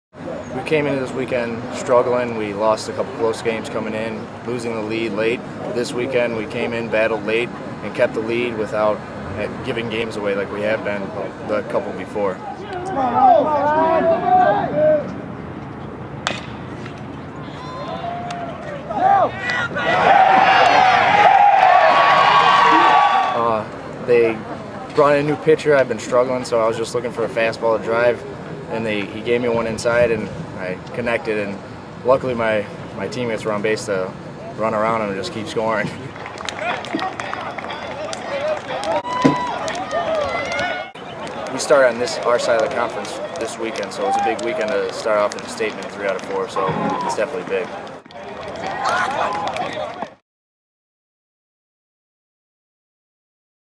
4 3 11 Lewis baseball vs Indianapolis
Lewis baseball splits with No. 25 Indianapolis. Lewis won game one 7-6. Indianapolis won game two 7-5.